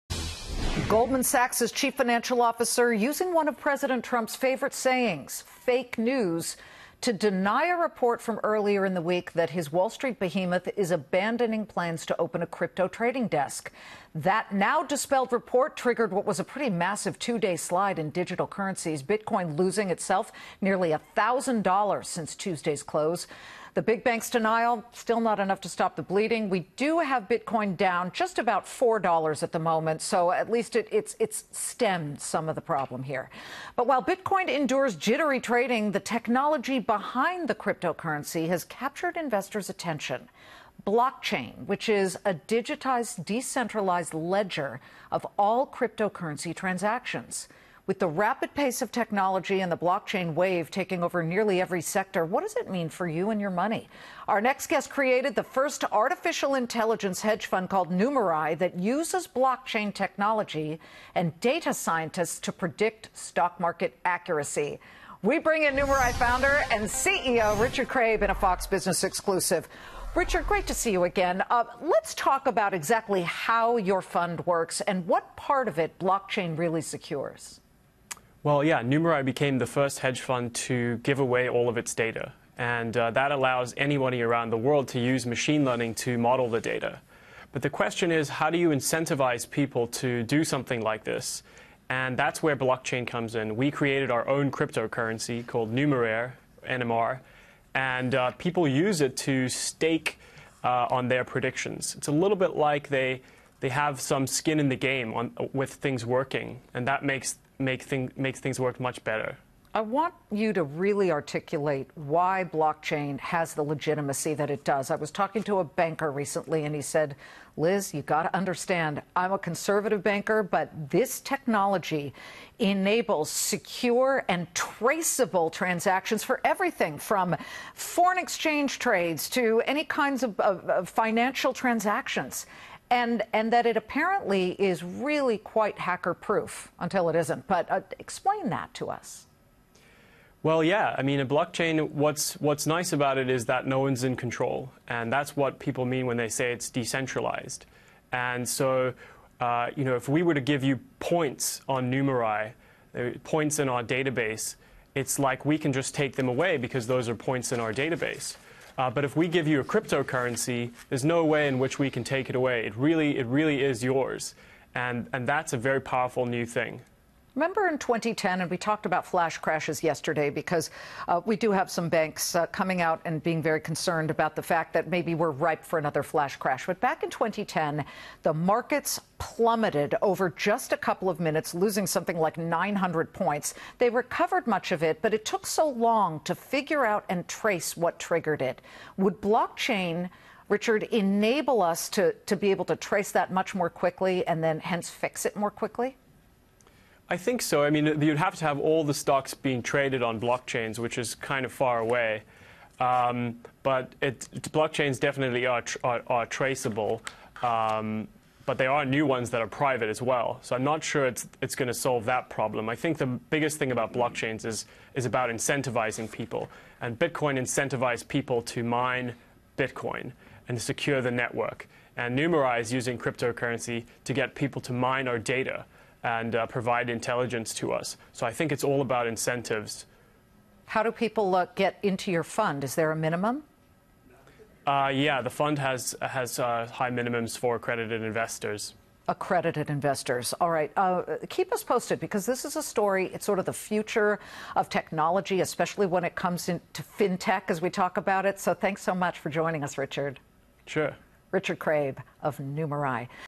AI hedge fund uses blockchain technology, data scientists to make trades
Numerai Founder Richard Craib discusses how his company uses blockchain technology and market scientists to predict stock market accuracy.